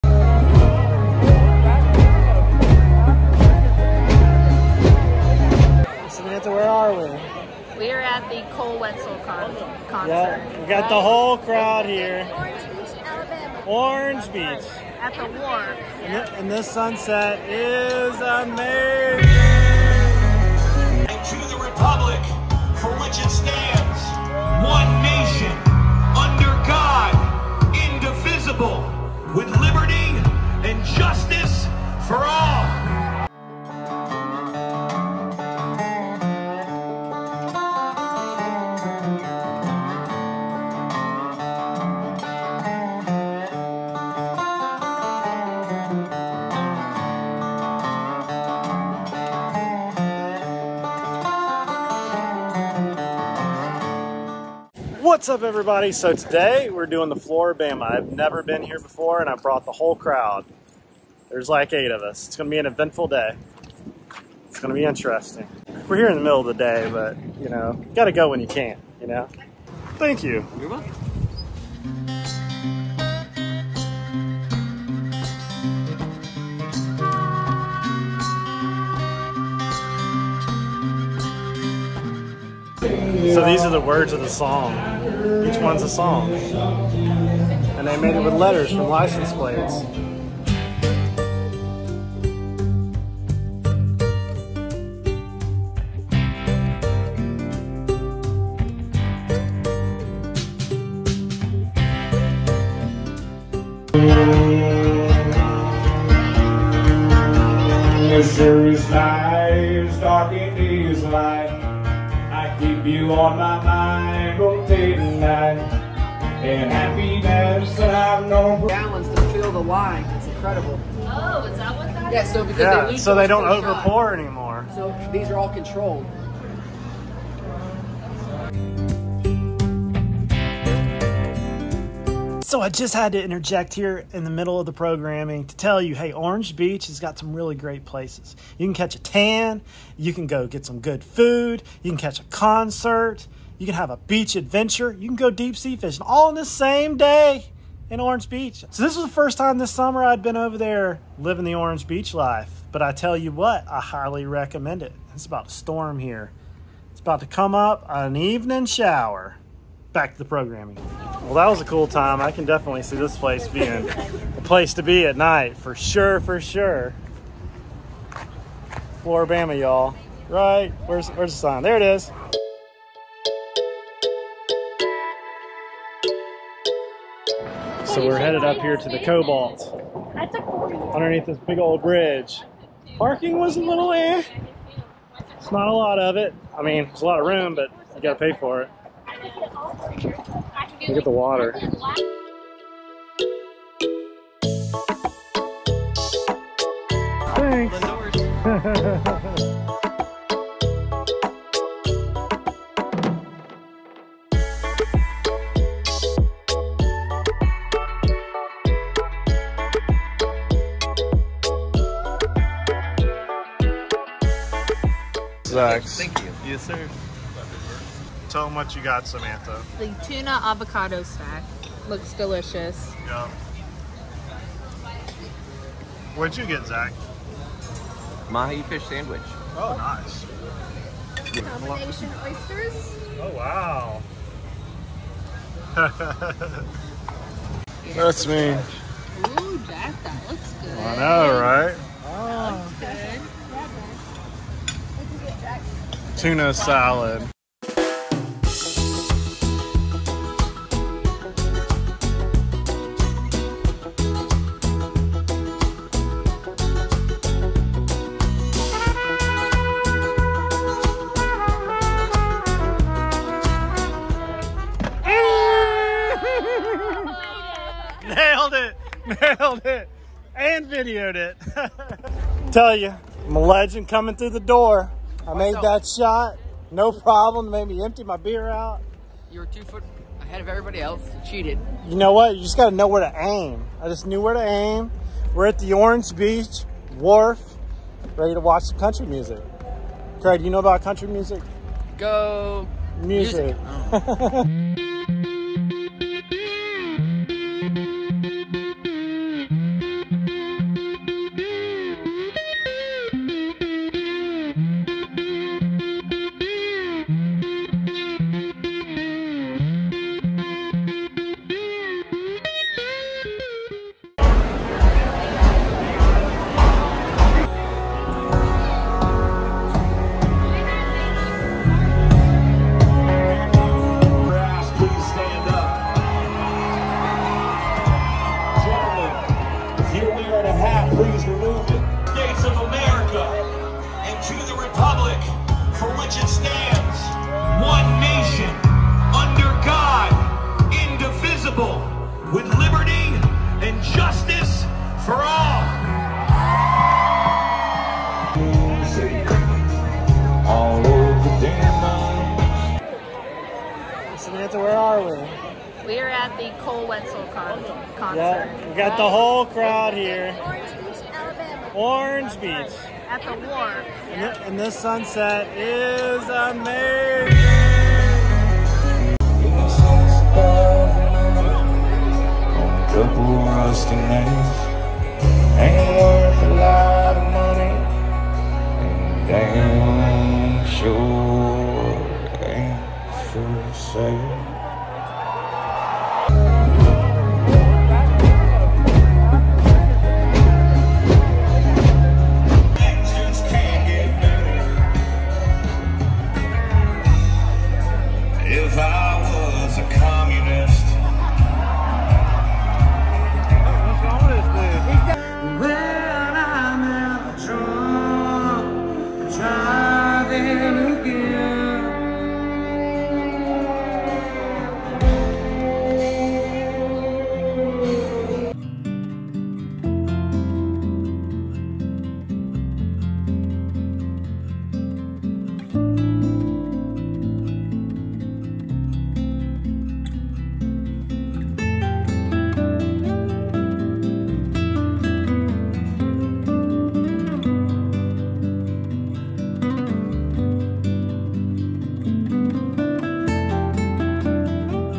The COOLEST places in Orange Beach, Alabama!
Here was our latest adventure to Orange Beach, Alabama. We wend to the Flora- Bama, Cobalt Restaurant and The Wharf for a concert!